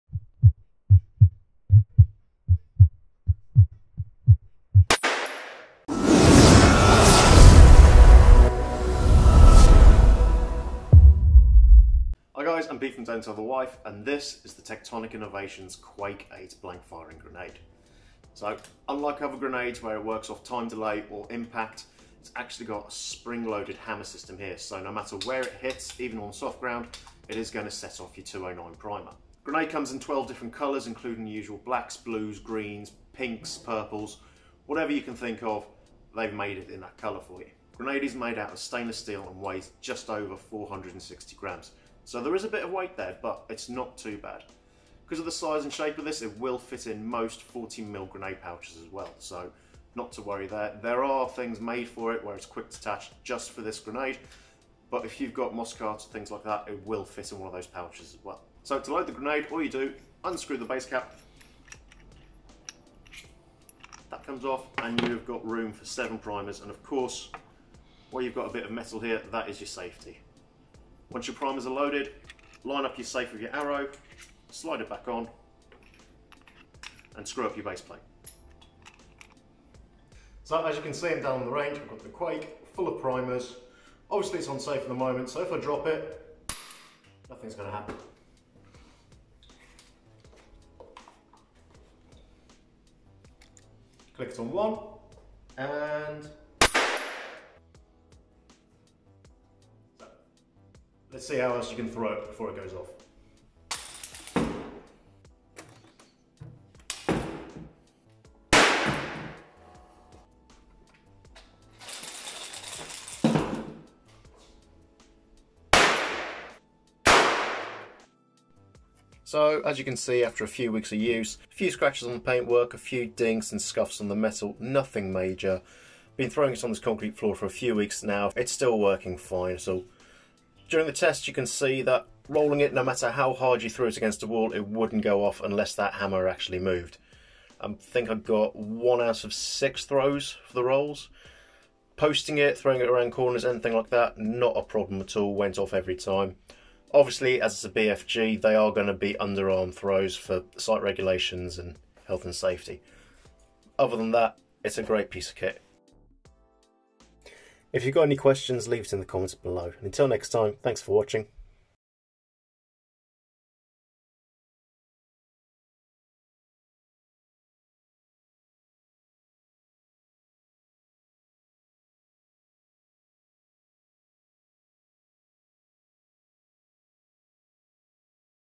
Tectonic Innovations Quake 8 impact grenade (Review)
This time we take a look at tectonic innovations quake 8 impact grenade designed for airsoft use and ideal for CQB game play. This airsoft grenade holds 7 .209 primers and allows you to easily change to a fresh blank in a second.